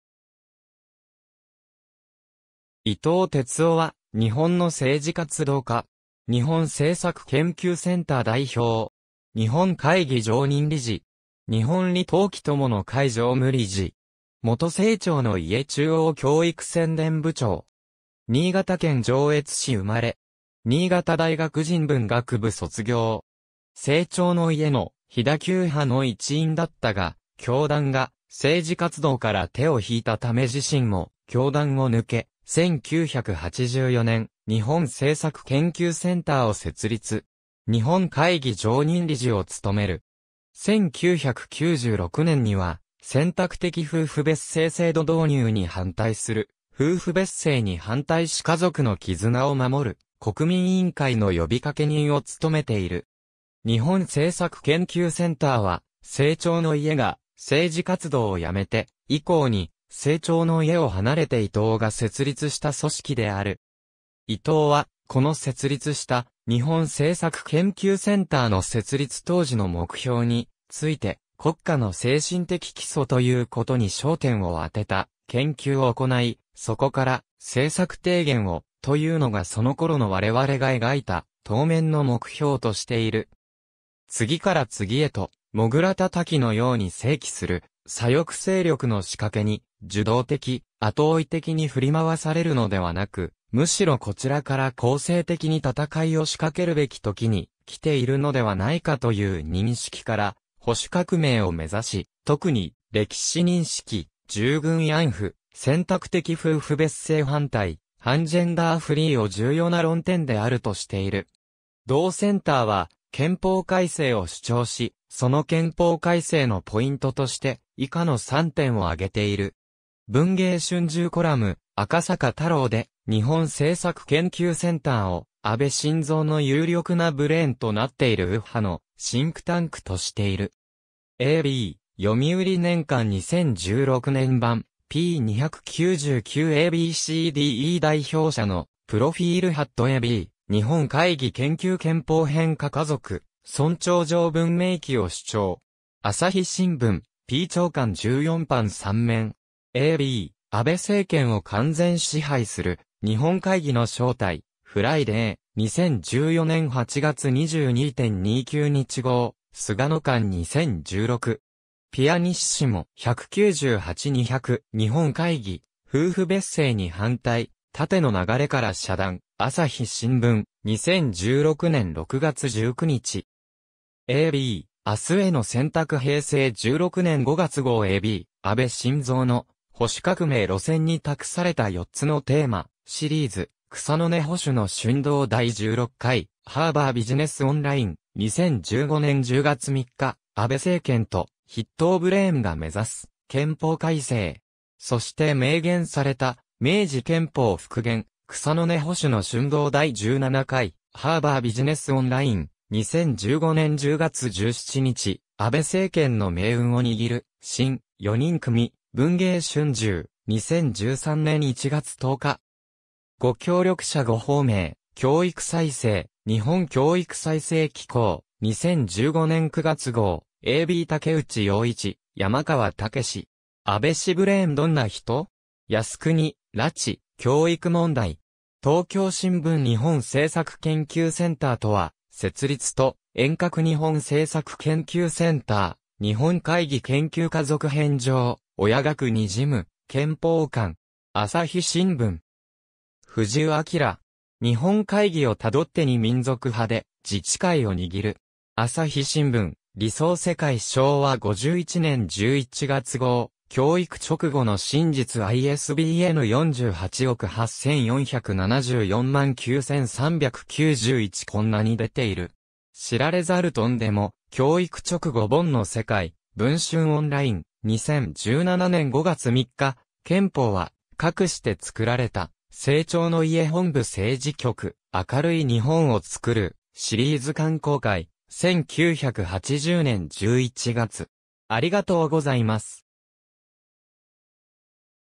伊藤哲夫 (政治活動家)
伊藤 哲夫（いとう てつお、1947年9月23日 - ）は、日本の政治活動家。
日本政策研究センター（任意団体）代表（自営）。
日本会議常任理事（政策委員）。
日本李登輝友の会常務理事。
元生長の家中央教育宣伝部長。
新潟県上越市生まれ。
新潟大学人文学部卒業。
生長の家の「飛田給派」（現・生長の家本流運動）の一員だったが、教団が政治活動から手を引いたため自身も教団を抜け、1984年、「日本政策研究センター」を設立。
日本会議常任理事を務める。
1996年には、選択的夫婦別姓制度導入に反対する「夫婦別姓に反対し家族の絆を守る国民委員会」の呼びかけ人を務めている。
日本政策研究センターは、生長の家が政治活動をやめて以降に生長の家を離れて伊藤が設立した組織である。
伊藤は、この設立した「日本政策研究センター」の設立当時の目標について、「『国家の精神的基礎』ということに焦点をあてた研究を行い、そこから政策提言を、というのがその頃のわれわれが描いた当面の目標」としている。
「次から次へとモグラたたきのように生起する左翼勢力の仕掛けに、受動的・後追い的に振り回されるのではなく、むしろこちらから攻勢的に戦いを仕掛けるべき時にきているのではないか」という認識から「保守革命」を目指し、特に、歴史認識、従軍慰安婦、選択的夫婦別姓反対、反ジェンダーフリーを重要な論点であるとしている。
同センターは、憲法改正を主張し、その憲法改正のポイントとして、以下の3点を挙げている。
文藝春秋コラム「赤坂太郎」で、日本政策研究センターを「安倍晋三の有力なブレーンとなっている右派のシンクタンク」としている。
^ a b 『読売年鑑 2016年版』（読売新聞東京本社、2016年）p.299 ^ a b c d e 代表者のプロフィール（日本政策研究センター） ^ a b “日本会議研究 憲法編 下 家族尊重 条文明記を主張”. 朝日新聞: p. 朝刊14版 3面. (2016年3月25日) ^ a b 「安倍政権を完全支配する『日本会議』の正体」、フライデー、2014年8月22・29日号 ^ 菅野完 2016,
pp. 198–200. ^ 「日本会議、夫婦別姓に反対　『タテの流れから遮断』」、朝日新聞、2016年6月19日。
^ a b 「明日への選択」平成16年（2004年）5月号 ^ a b 「安倍晋三の『保守革命』路線に託された4つのテーマ――シリーズ【草の根保守の蠢動 第16回】」、Harbor Business Online、2015年10月3日 ^ 「安倍政権と筆頭ブレーンが目指す『憲法改正』。そして明言された『明治憲法復元』【草の根保守の蠢動 第17回】」、Harbor Business Online, 2015年10月17日 ^ 「安倍政権の命運を握る『新・四人組』」、文藝春秋、2013年1月10日
^ 「ご協力者御芳名」、『教育再生』、日本教育再生機構、2015年9月号 ^ a b 竹内洋一、山川剛史 (2006年9月9日). “「安倍氏ブレーン」どんな人？靖国、拉致、教育問題…”. 東京新聞 ^ 日本政策研究センターとは 設立と沿革 日本政策研究センター ^ “日本会議研究 家族編 上 「親学」にじむ憲法観」”. 朝日新聞. (2016年6月17日) 。
^ 藤生明 (2016年11月9日). “日本会議をたどって 2 民族派で自治会を握る”. 朝日新聞 ^ 『理想世界』昭和51年（1976年）11月号 ^ 『教育勅語の真実』（致知出版社、2011年）ISBN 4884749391 ^ こんなに出ている！　知られざるトンデモ「教育勅語」本の世界、文春オンライン、2017年5月3日 ^ 「憲法はかくして作られた」、生長の家本部政治局（編）、明るい日本を作るシリーズ刊行会、1980年11月。